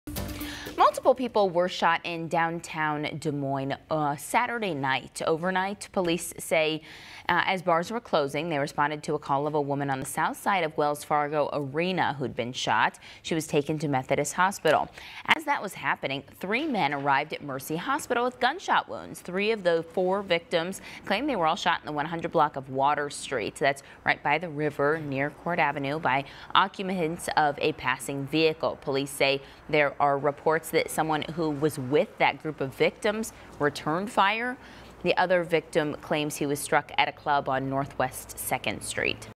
Multiple people shot in downtown Des Moines overnight, police say
At least four people were hospitalized early Sunday morning, all with survivable injuries, according to police.

Des Moines police said around bar closing time, they received a call of a woman suffering a gunshot wound near the south side of Wells Fargo Arena. She was transported to Methodist Hospital.